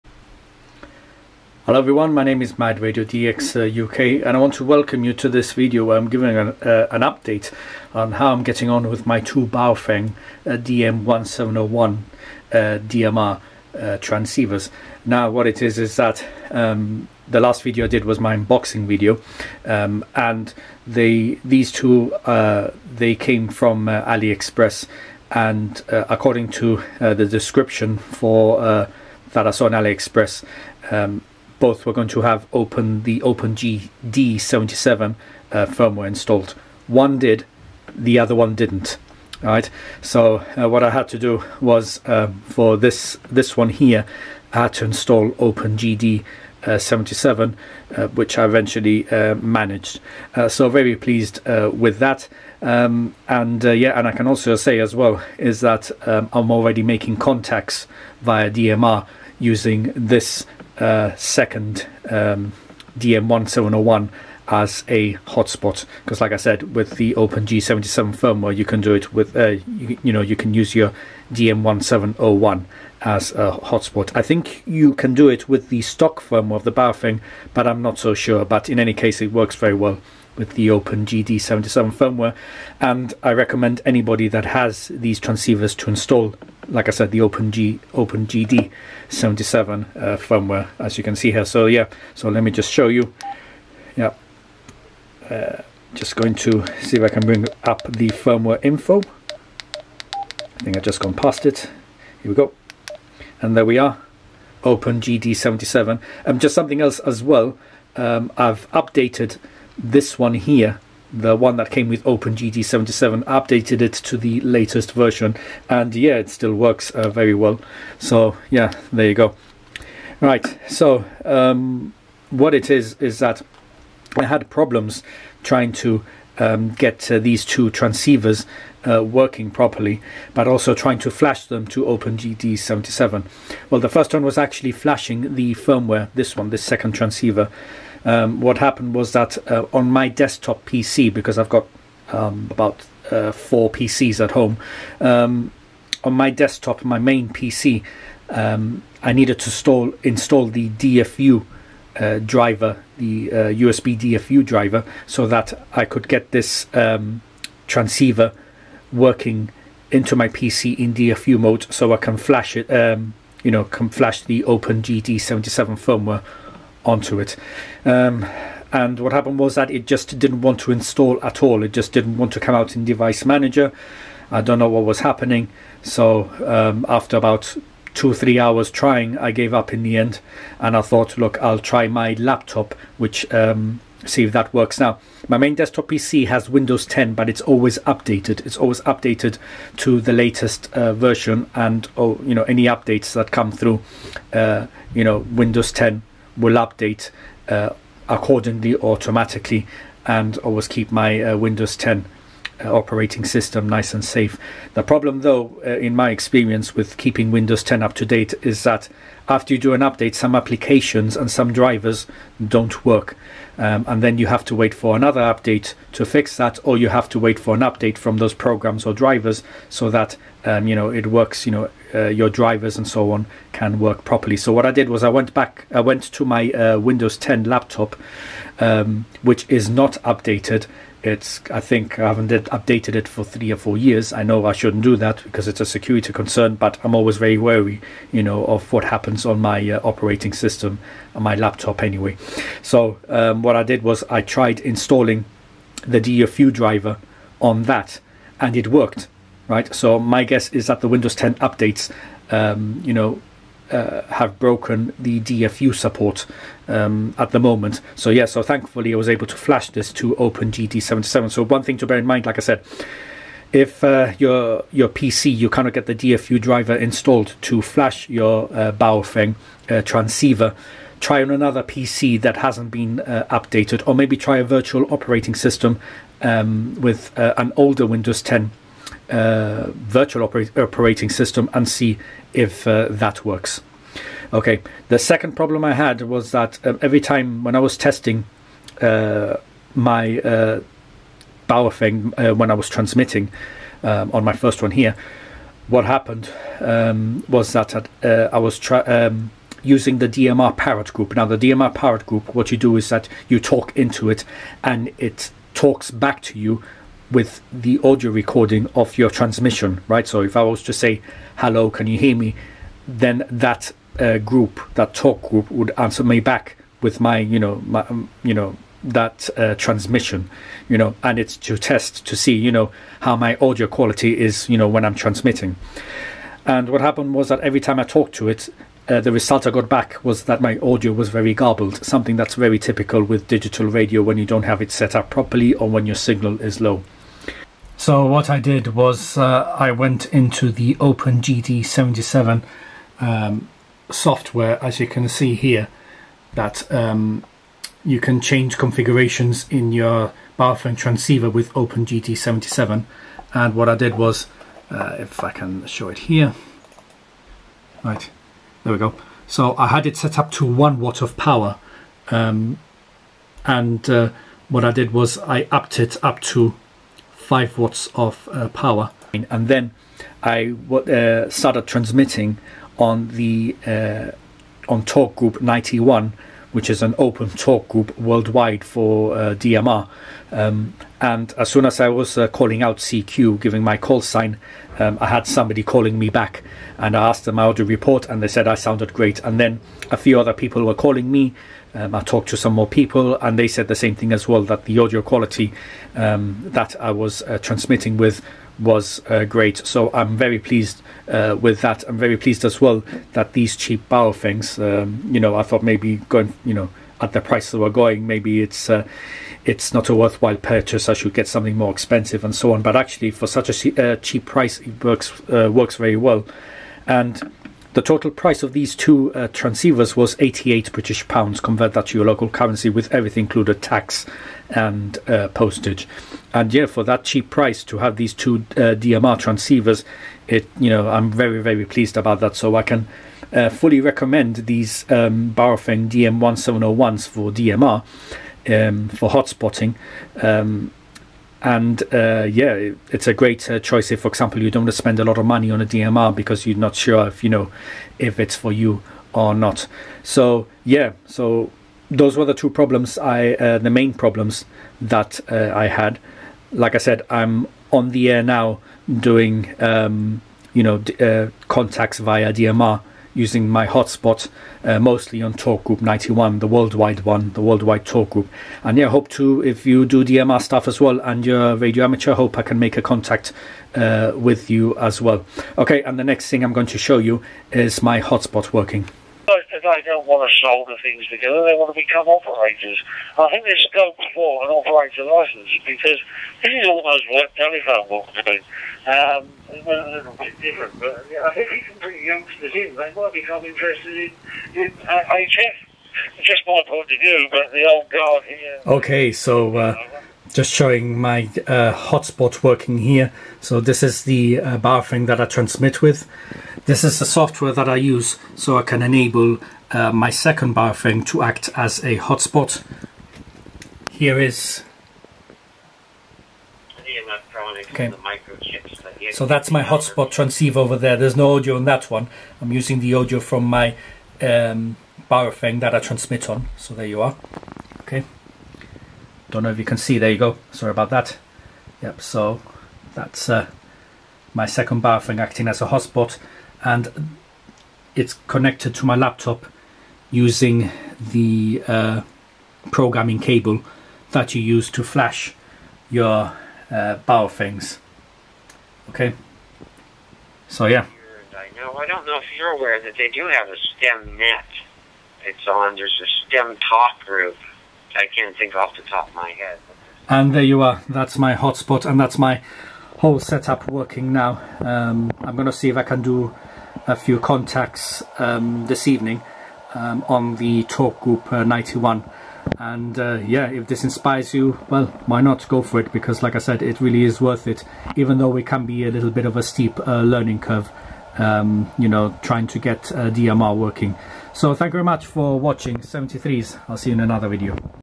Baofeng DM-1701 for DMR: 2nd day, how am I getting on & Hotspot already working
Showing my progress with my Baofeng DM-1701 transceivers on my 2nd day. I also show my Hotspot setup working which has enabled me to make contacts already.
Something to note in this video, although setting up my Hotspot for 5 watts seems extreme for the home, this solved the garbled audio issue I had on transmit. My guess is that I have a noise issue at home on the 70cm band so therefore bumping up the power enables me to transmit properly past the noise. Each person's mileage will vary, some can get away with 0.25 watts of power, others won't like myself.

00:00 Introduction
00:15 Why I am giving an update?
00:53 Already making DMR contacts via hotspot
01:22 Recommending the OpenGD77 firmware
02:01 Problem with DFU driver installation on Windows 10
04:25 Advice for DFU driver installation issues
04:44 2nd problem, bad audio on transmit
05:42 My solution to bad audio
06:11 I was finally able to talk to other operators
06:44 Pleased with the two Baofengs
07:13 Recommending the Baofeng DM-1701 for DMR
07:58 Showing my hotspot working & setup
09:32 Recommending DMR